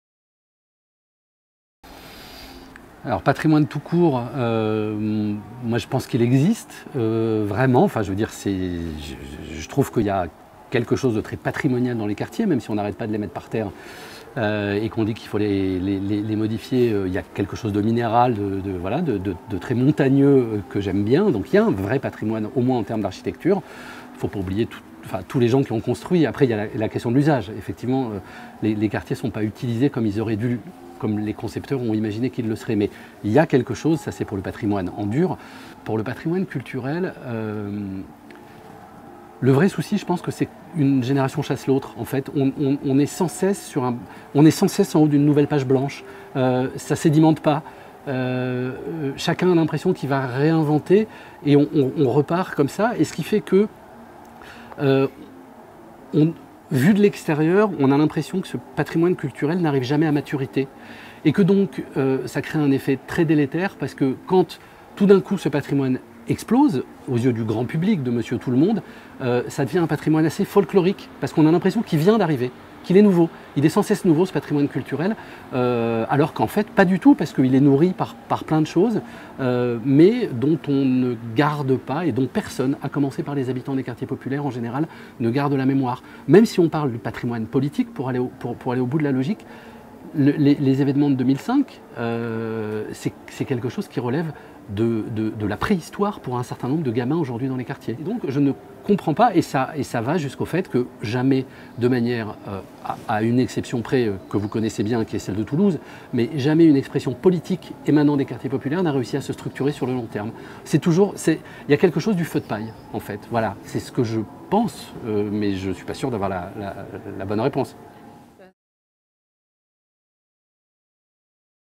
Entretien avec Edouard Zambeaux, journaliste (mai 2017)
Entretien réalisé à l’occasion du colloque « Un siècle d’histoire et de mémoire. Enjeux patrimoniaux et artistiques de l’immigration et des outre-mer »
Mardi 23 et mercredi 24 mai 2017 à l’Institut des cultures d’islam
Evénement co-organisé par le Groupe de recherche Achac et Tactikollectif.